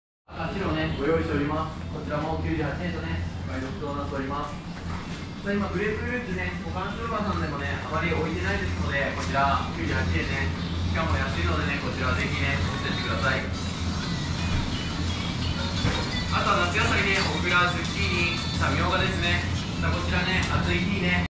inside a japanese grocery store (Tokyo)
my first day in tokyo, japan
wandering around  (in the intense summer heat!) with my go pro